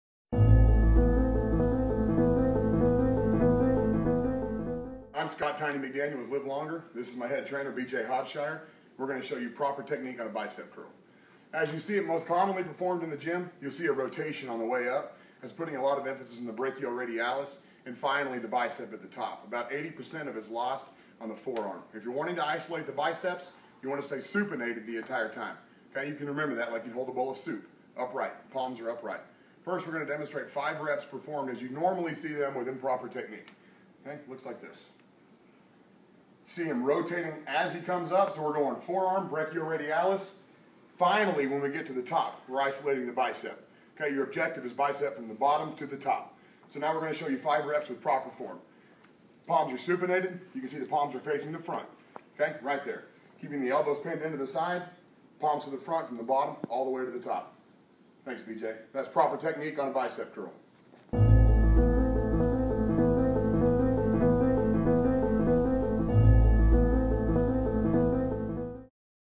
LIVE LONGER: BICEP CURLS
Scott "Tiny" McDaniel explains proper technique on how to do a Bicep Curl.
Operation: LiveLonger MMXIII --
Accomplishing your health related New Years Resolutions in 90 days or less!! Join the revolution!